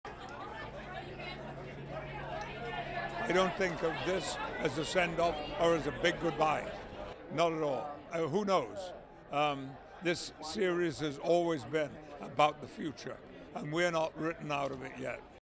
Patrick Stewart explains why he doesn't see the final season premiere of "Star Trek: Picard" as A go
ASSOCIATED PRESS
Los Angeles, 9 February 2023
1. Wide shot cast posing at Season 3 premiere of "Star Trek: Picard"
2. SOUNDBITE (English) Patrick Stewart, actor :
"I don't think of this as a send off or as a big goodbye. Not at all. Who knows? This series has always been about the future. And we're not written out of it yet."